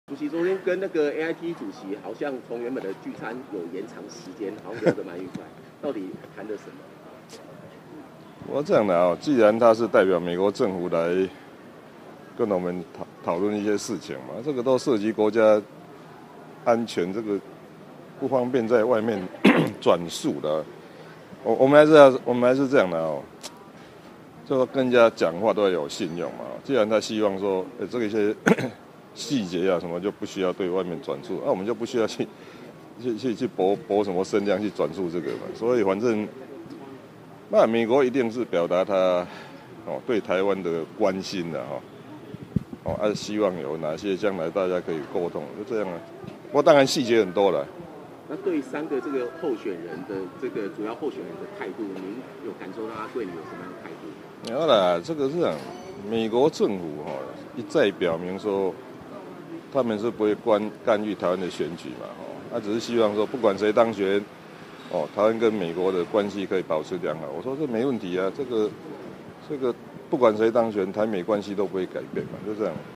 柯文哲昨會羅森伯格 尊重對方不透露細節【政治快訊】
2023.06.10【政治快訊】柯文哲今（10）日參訪鳳山天公廟，接受媒體聯訪，被問及昨日會面羅森伯格，帶您了解。